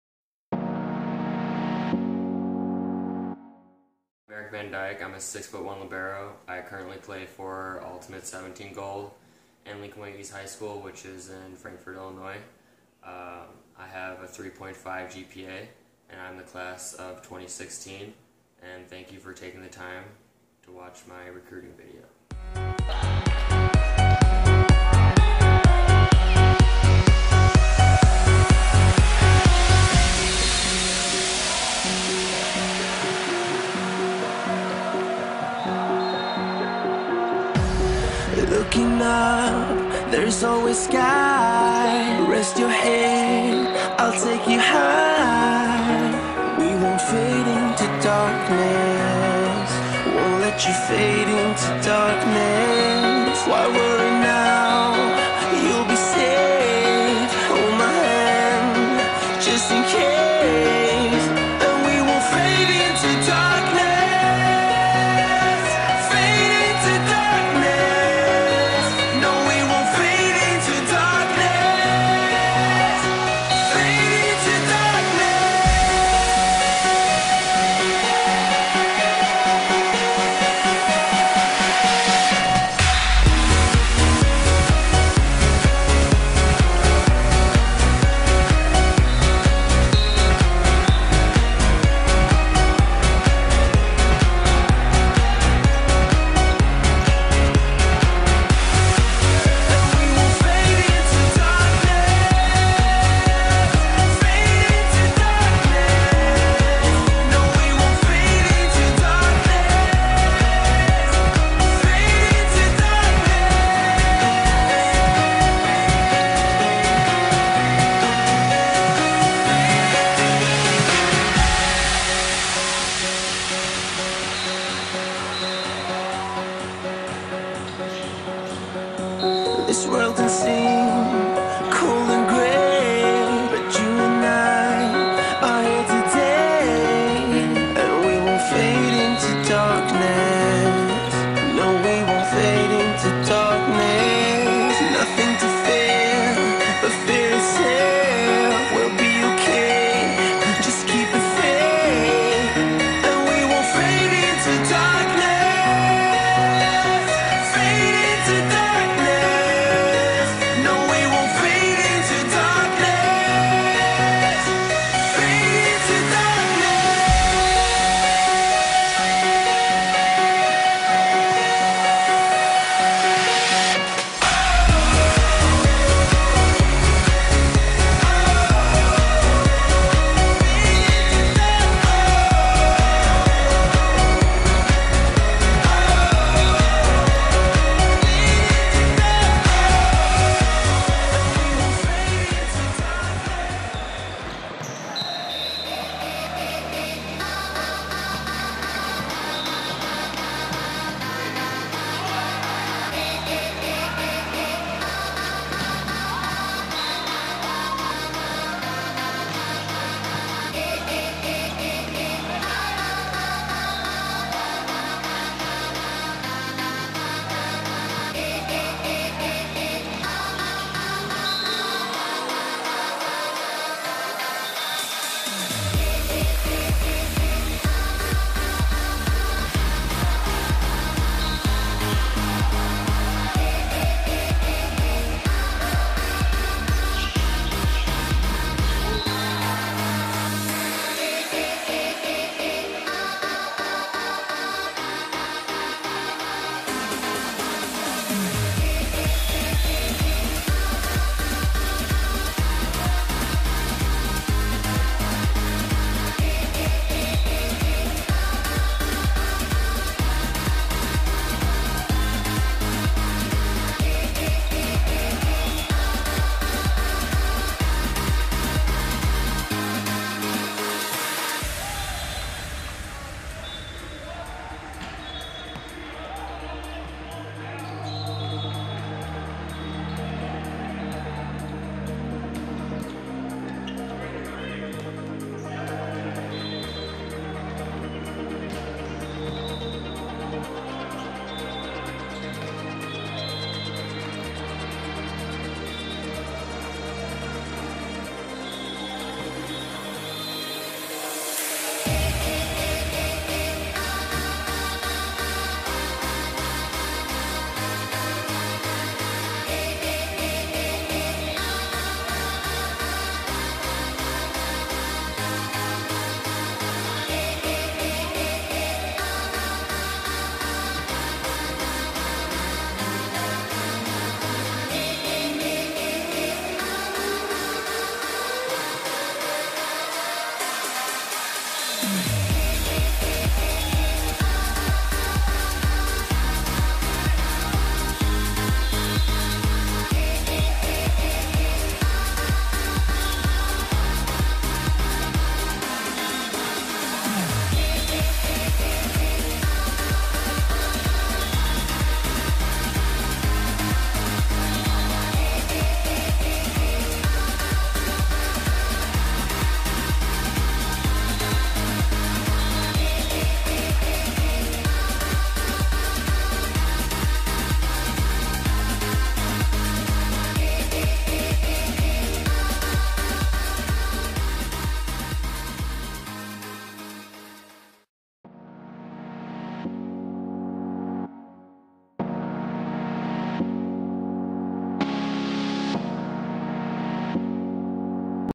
Eric Van Dyck Libero Volleyball Recruitment Video Class of 2016
3.5 GPA
6ft 1in
Libero
Ultimate 17 Gold #33
Lincoln-Way East #1
2015 USA CONTINENTAL TEAM
2015 USA A2 TRAINING CAMP